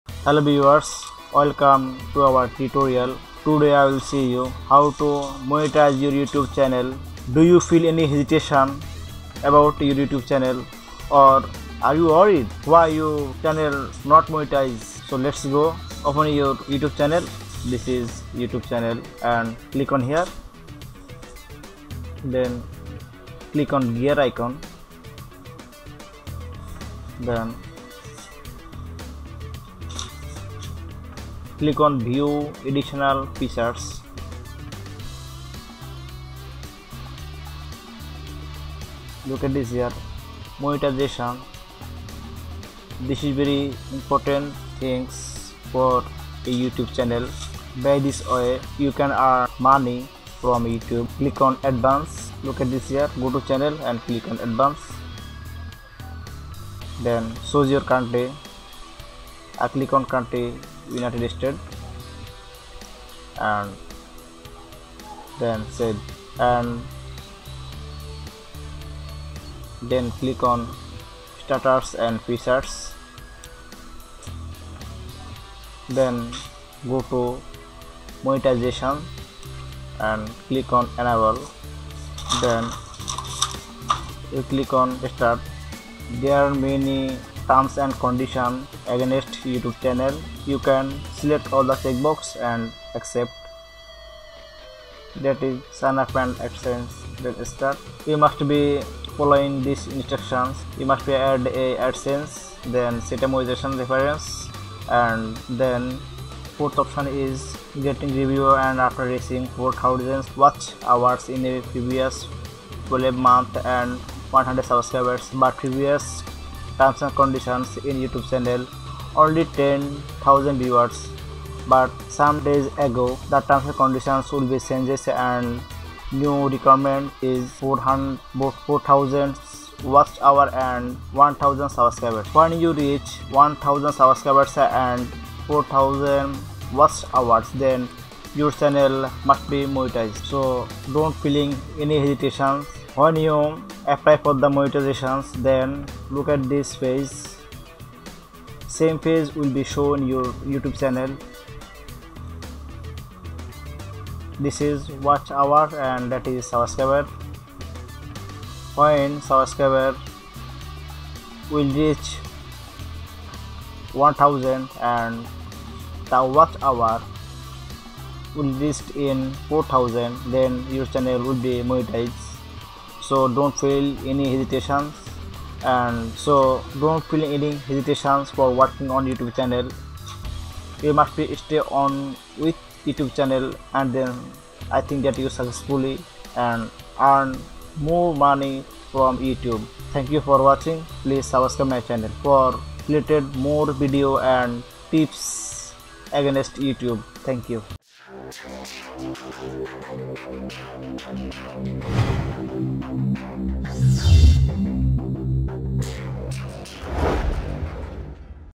YouTube New Monetization Rules 2018 | New Guidelines for YouTube
This is a YouTube New Monetization Rules 2018 | New Guidelines for YouTube Tutorial video, in this video we try to show How to

monetize your youtube channel.when you see the video you can easily done it.